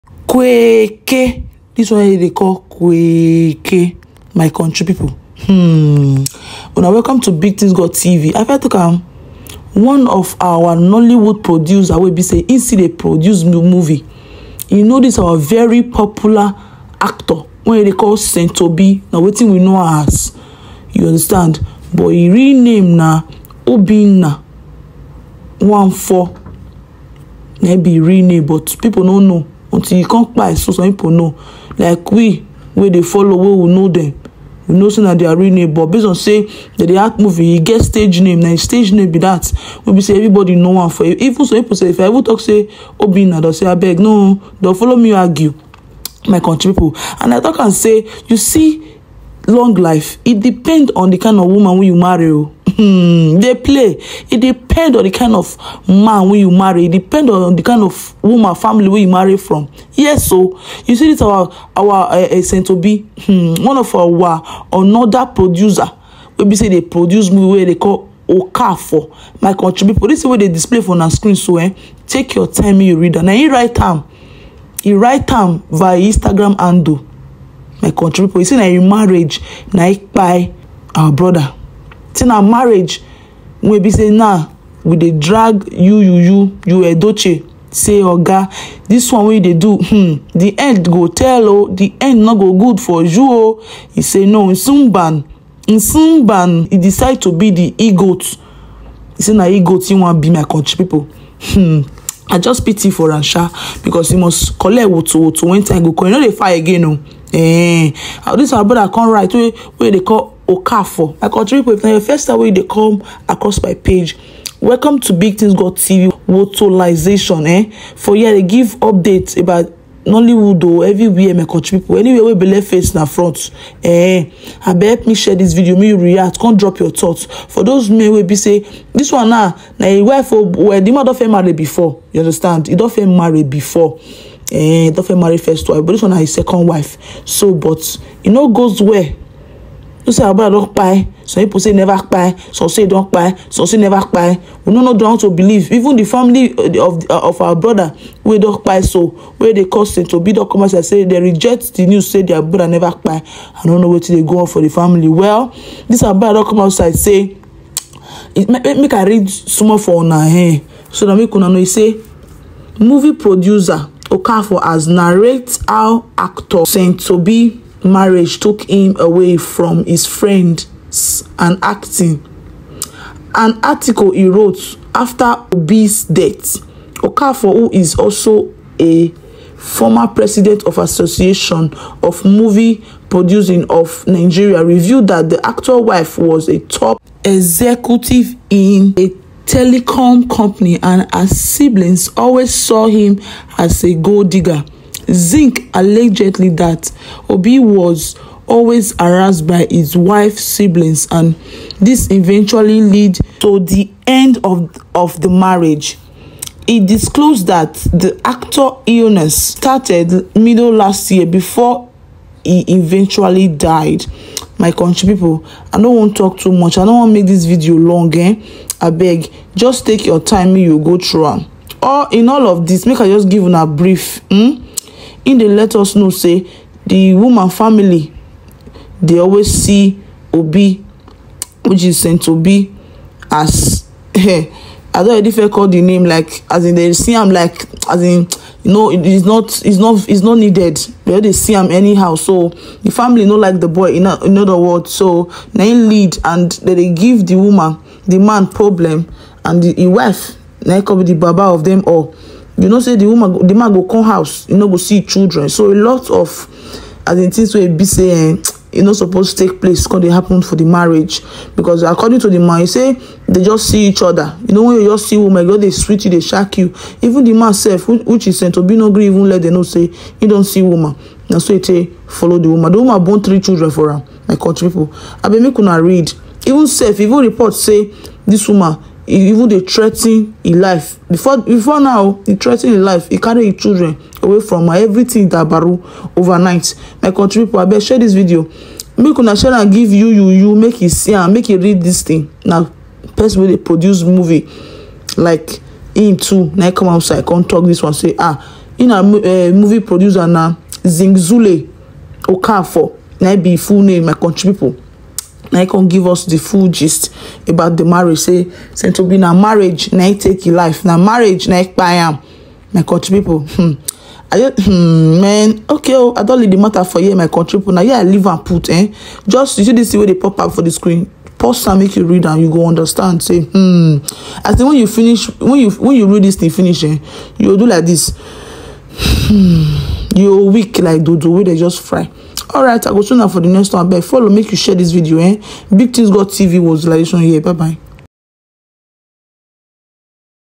Movie producer Zik Zulu okafor has narrated how actor Saints Obi’s marriage took his life
Former president of Association Movie producer Zik Zulu okafor has narrated how actor Saints Obi’s marriage took his life